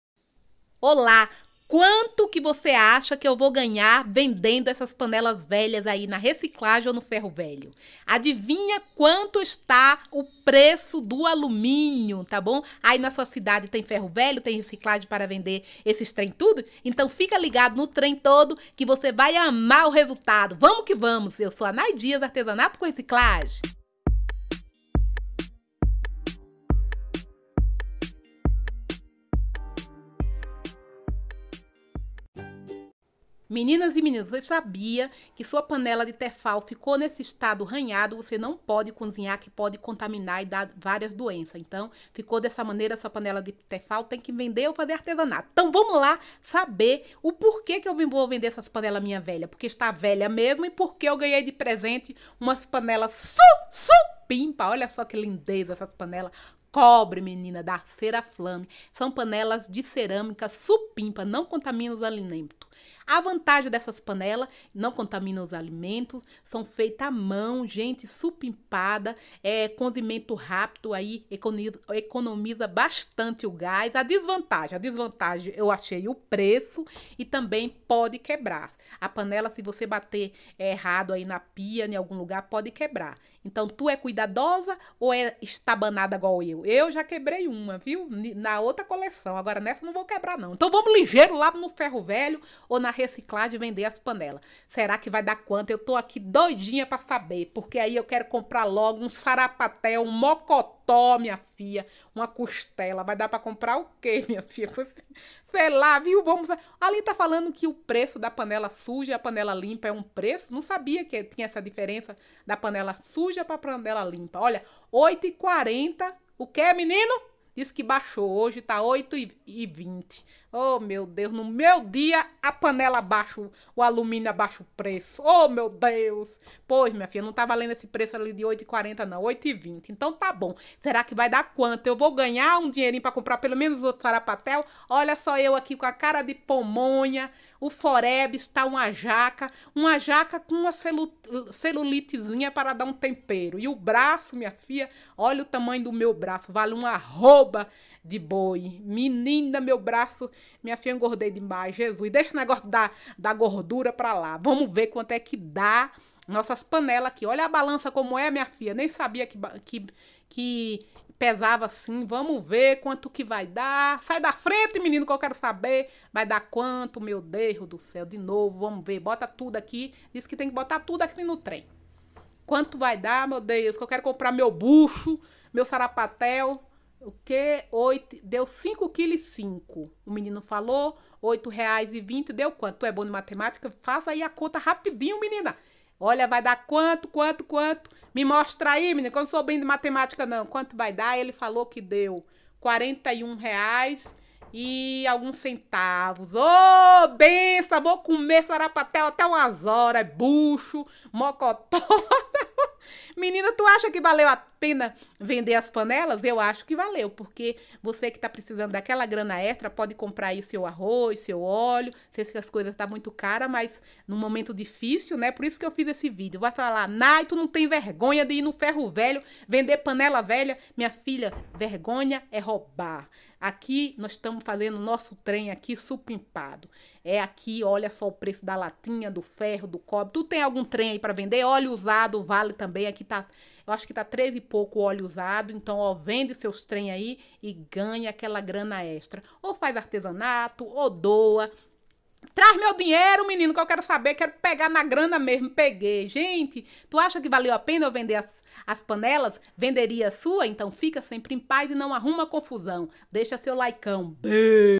VOCÊ JOGA FORA PANELAS VELHAS? IDEIAS DE RECICLAGEM.
Ideias de Reciclagem de Panelas velhas. Dica e ideias de Ganhar uma grana Extra 💰💰

Material
Panelas velhas
e MUITA DISPOSIÇÃO PARA SAIR DE CASA PRA VENDER SUAS PEÇAS VELHAS NO FERRO VELHO

* Tekbond (Colas e Adesivos)
* Vasap Designer (Vasos Sustentável)

🙋👉🙇Divulguem o vídeo para seus amigos :)💯